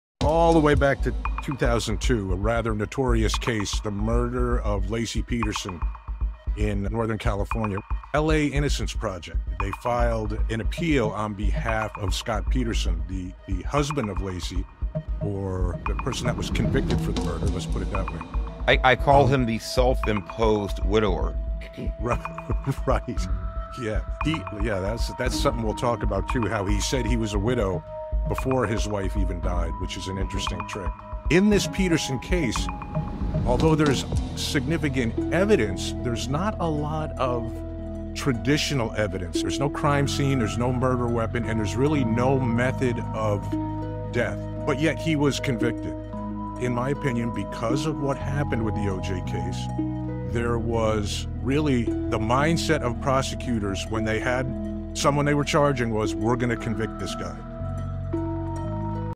The 2002 Murder of Laci Peterson: LA Innocence Project files Appeal for Convicted Murderer... Why?
The 2002 Murder of Laci Peterson: LA Innocence Project files Appeal for the convicted Murderer... Why now?

On Christmas Eve 2002, Scott Peterson reported his wife, Laci Peterson, missing.  She was 27 years old and eight months pregnant.  In April 2023, Laci's body was discovered and a short time later, Scott Peterson was charged with her murder.  Scott was convicted, and eventually sentenced to life in prison.  Twenty plus years later, the Los Angeles Innocence Project filed an appeal on Peterson's behalf claiming factual innocence.  They raise four specific points which they claim were not sufficiently investigated because they didn't support the police department's theory of the crime.

New Episode is Live on all platforms